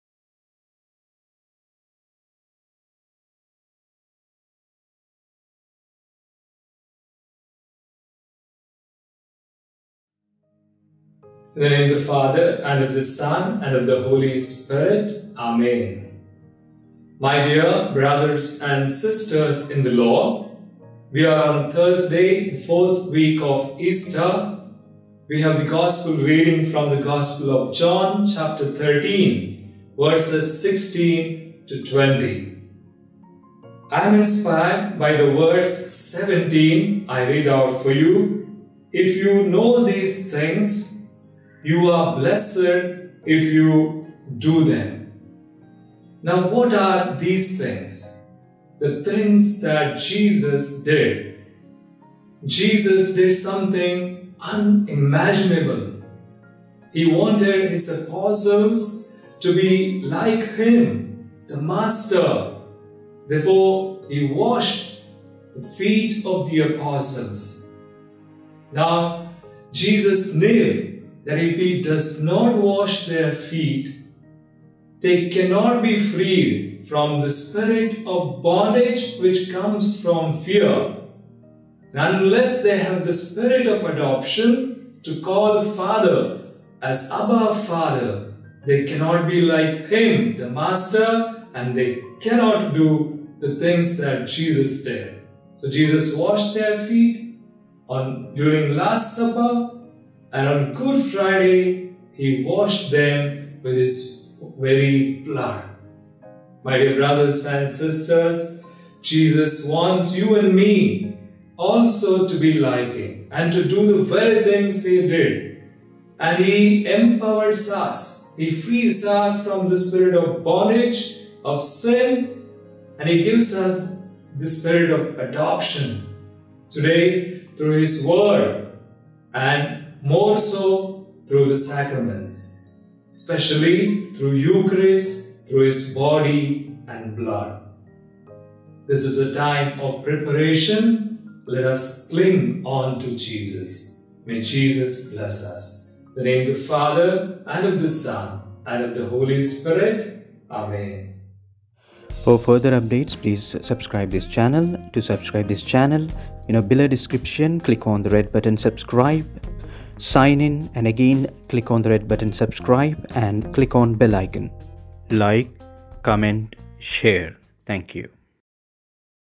A short reflection on the Gospel of John 13:16-20 (Thursday 4th Week of Easter 7th May 2020)
A short reflection on the Gospel of John 13:16-20 (Thursday 4th Week of Easter 7th May 2020) by Fr. Anil kiran SVD
 St. Theresa's Church, Bandra West.